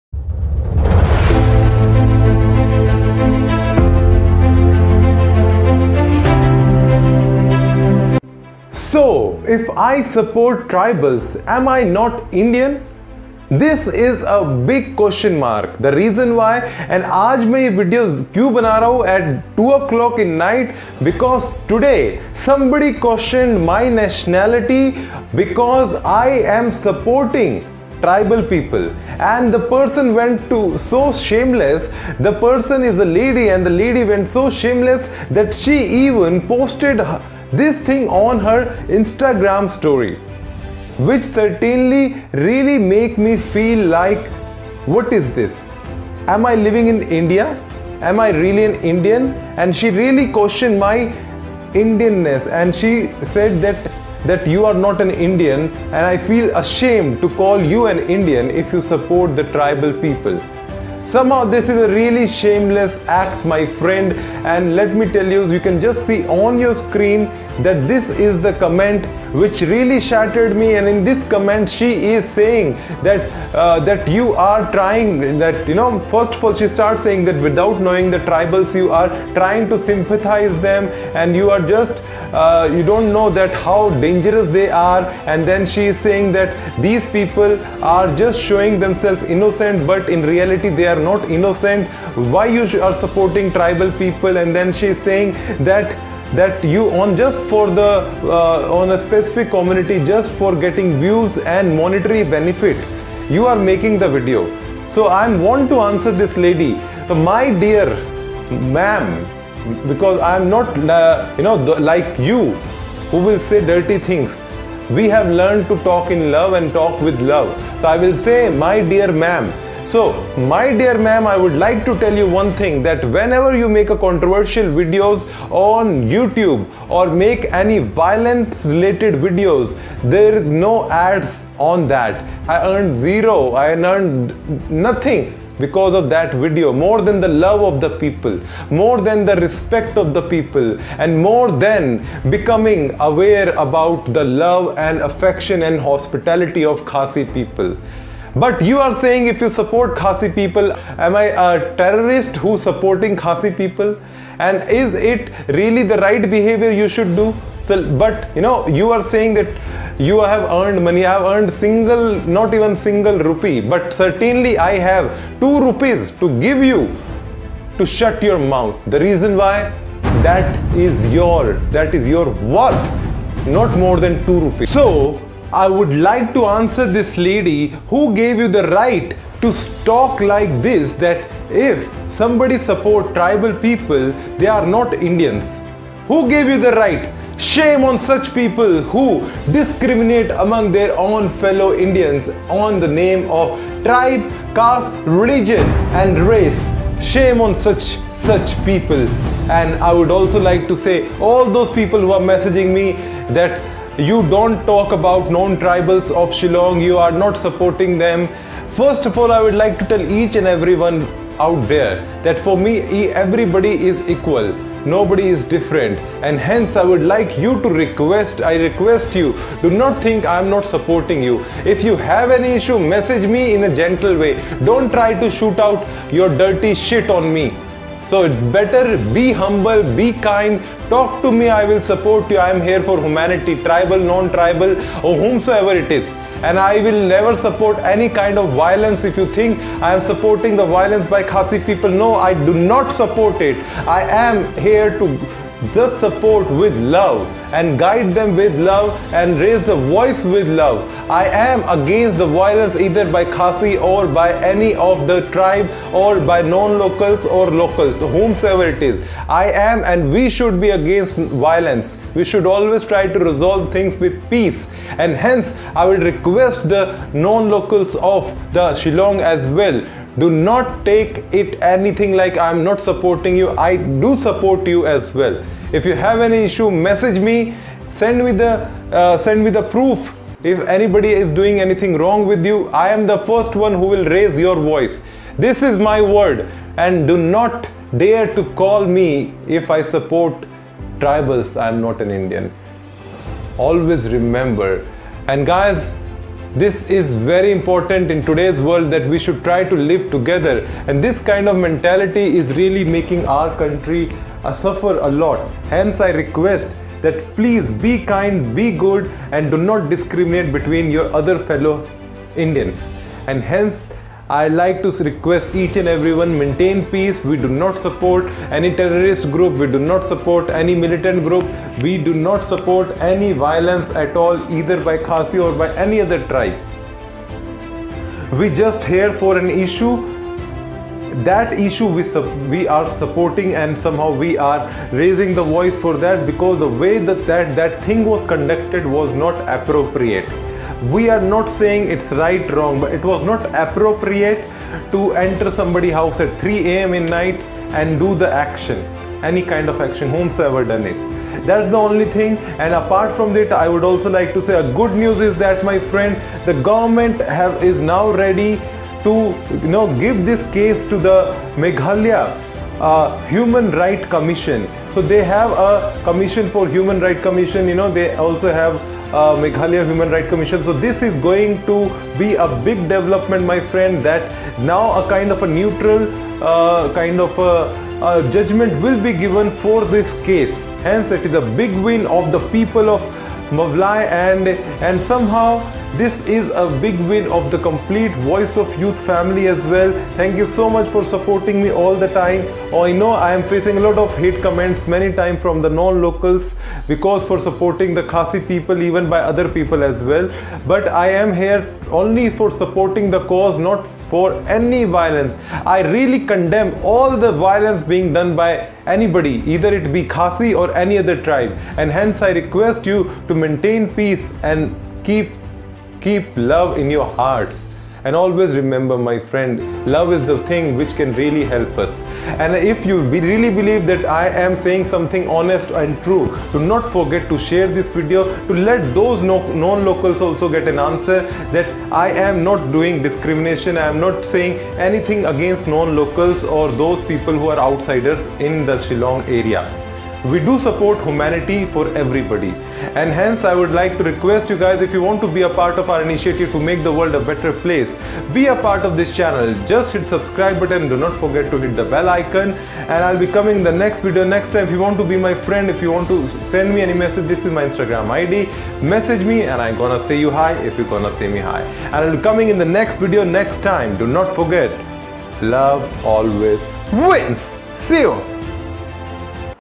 Am i not an Indian? if i support Tribals | am i ignoring Non Tribals ? Good news for Khasi Justice
Here i reply to those haters who are against justice for khasi.